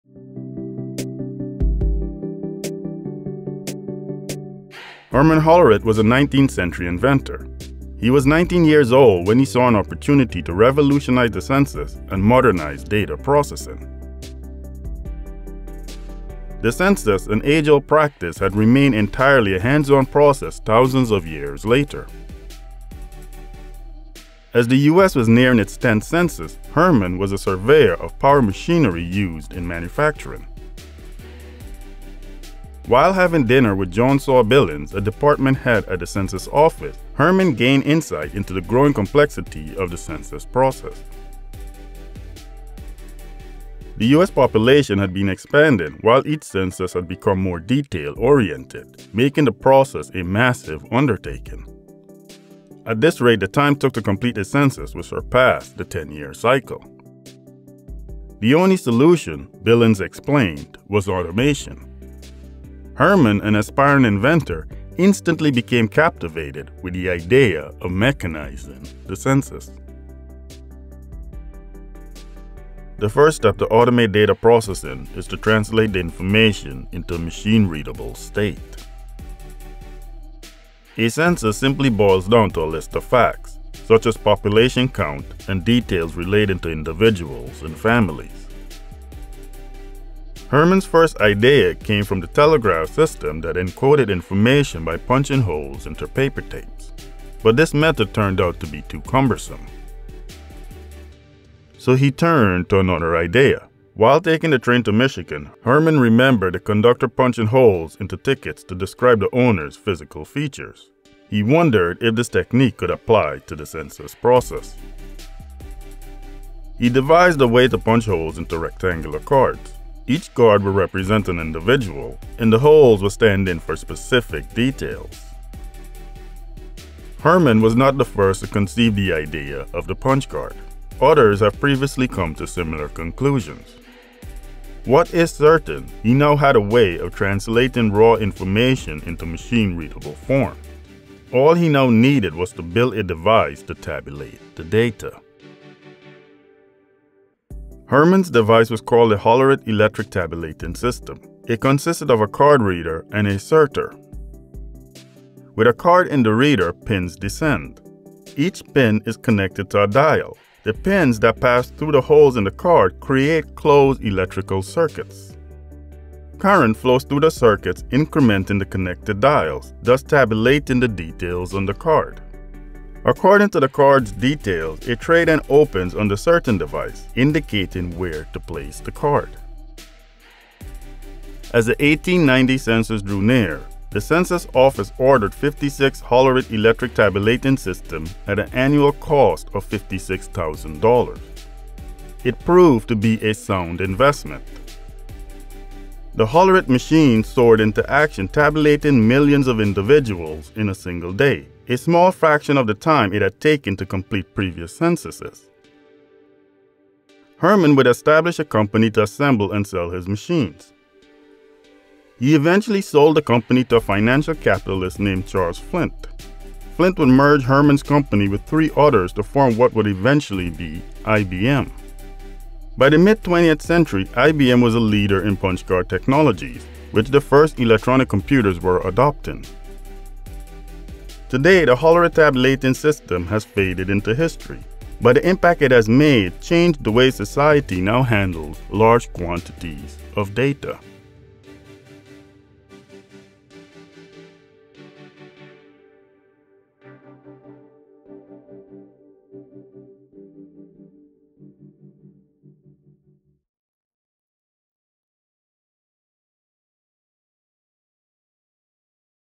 The invention of modern data processing
While taking a train to Michigan, he remembered the conductor punching holes into tickets to describe the owner's physical features. He wondered if this technique could apply to the census process.